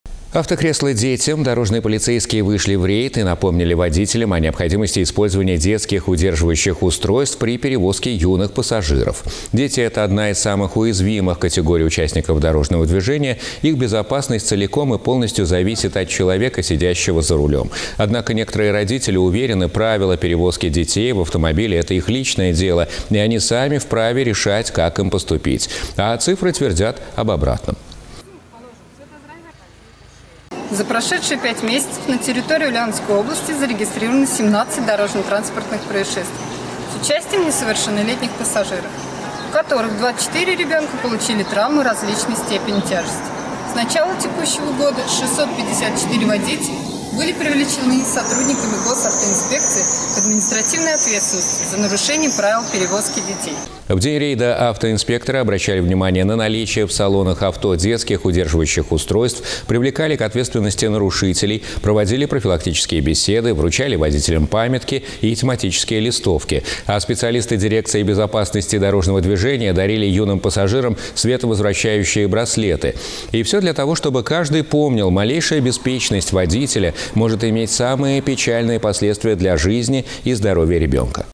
В Ульяновске прошла акция ГАИ "Автокресло детям"
Сотрудники дорожной полиции провели рейд и напомнили водителям о важности использования детских удерживающих устройств при перевозке детей.